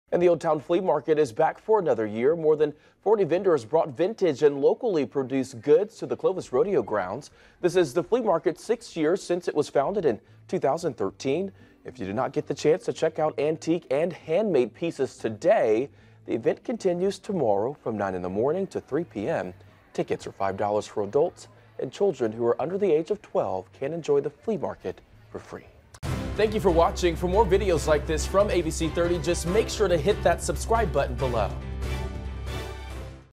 Clovis flea market
More than 40 vendors brought vintage and locally produced goods to the Clovis Rodeo Grounds.